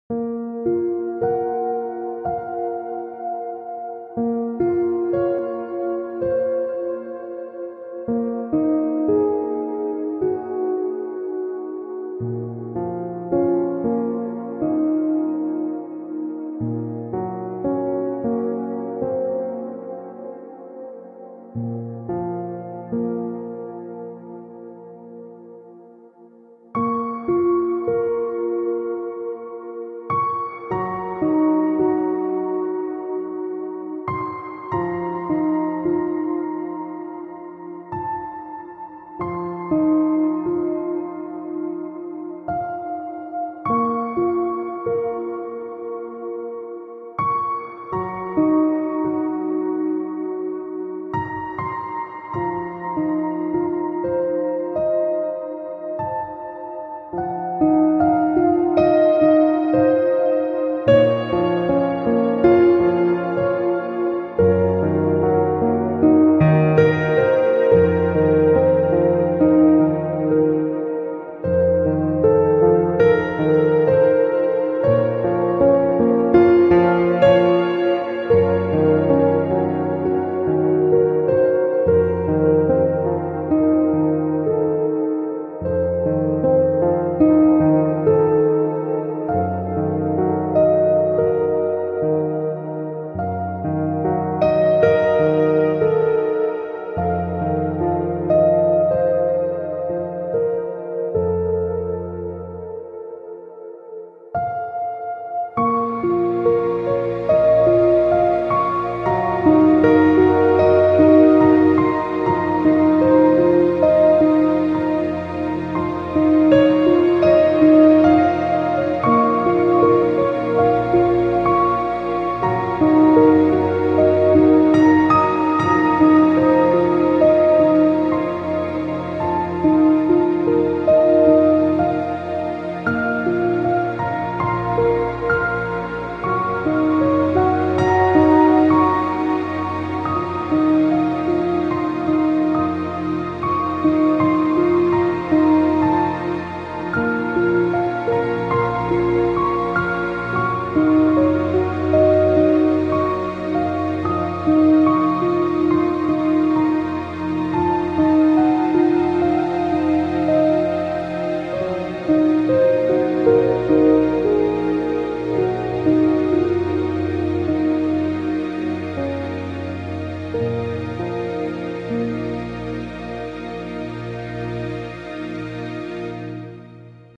Byzantine icon painting
My first video with the process of painting an icon from start to finish🎨